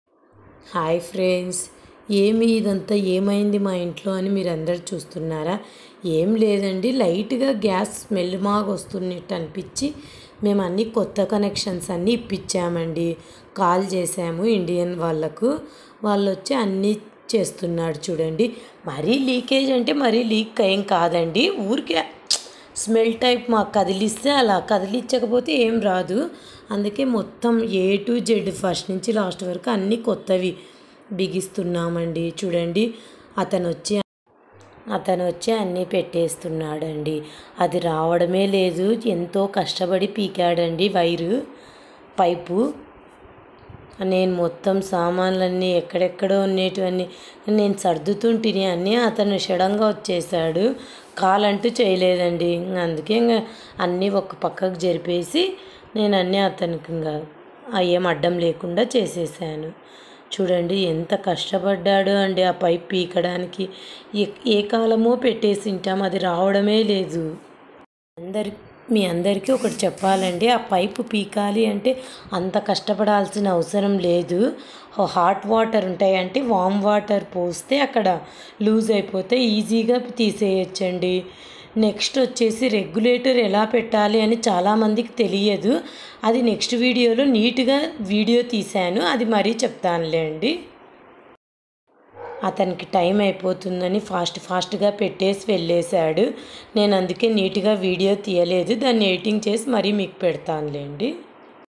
అయ్యో 🔥మా ఇంట్లో గ్యాస్ లీకేజీ అయింది అండి చిన్న ప్రాబ్లమ్|gas cylinder leakage fire problem sefty
gas leakage problem